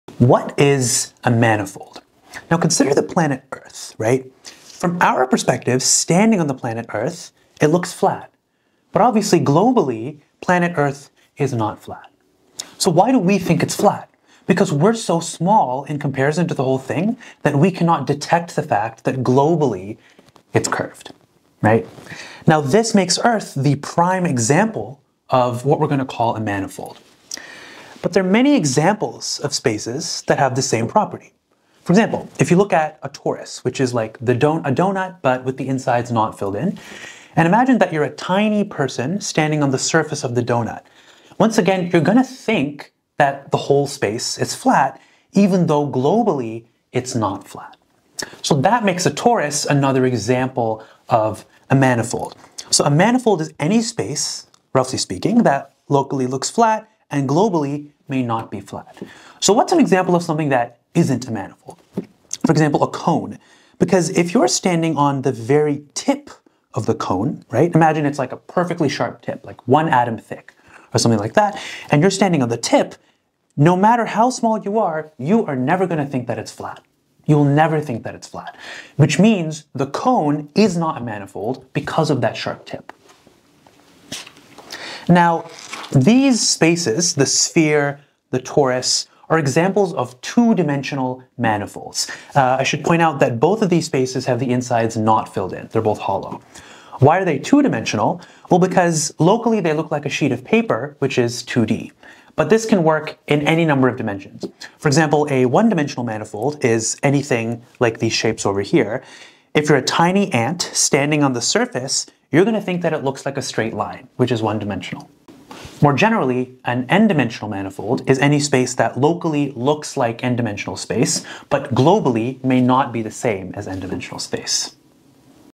Manifolds, explained intuitively
A high-level explanation of what a manifold is.